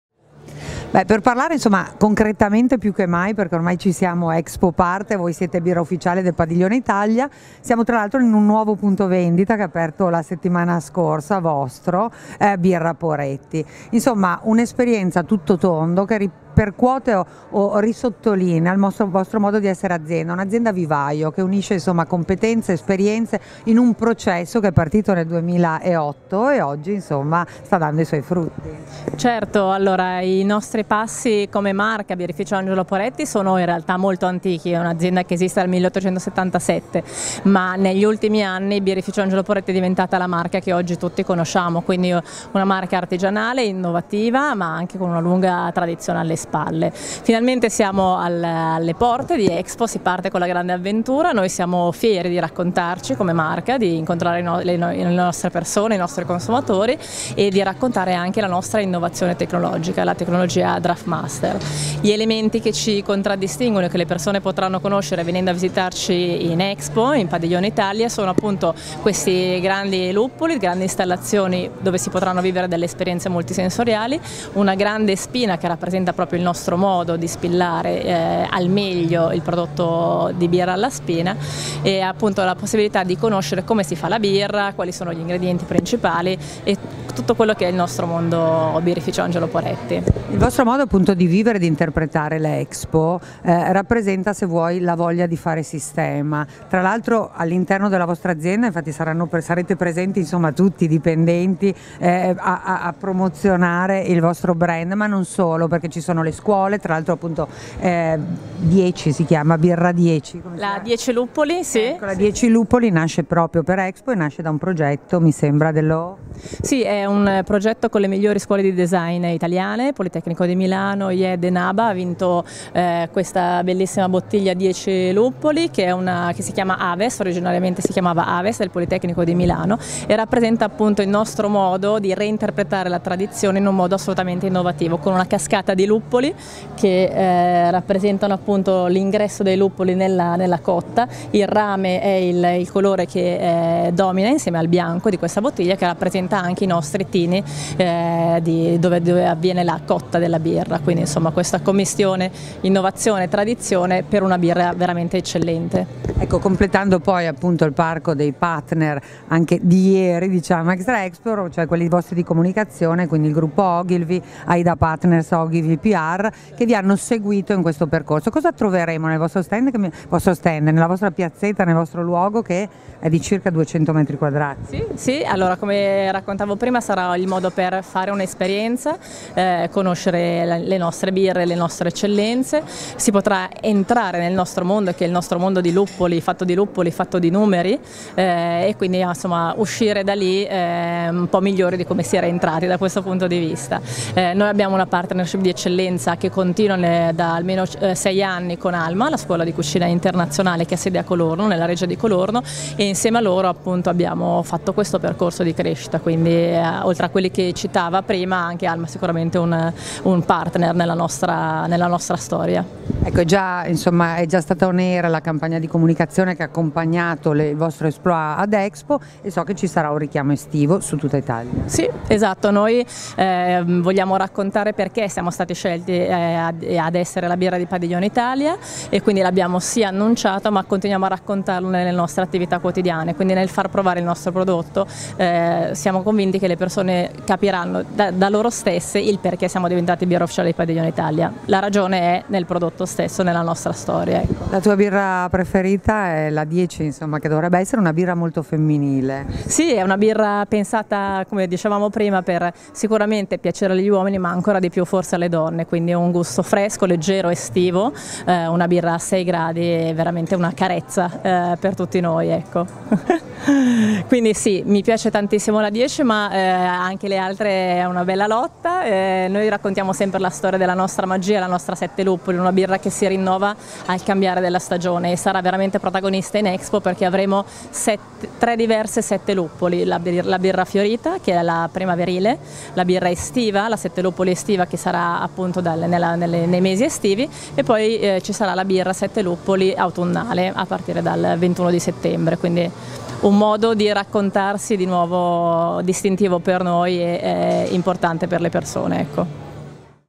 Birrificio Angelo Poretti, birra ufficiale di Padiglione Italia a Expo 2015
youmark.it - Intervista a Manuela Rognoni, marketing manager Carlsberg Italia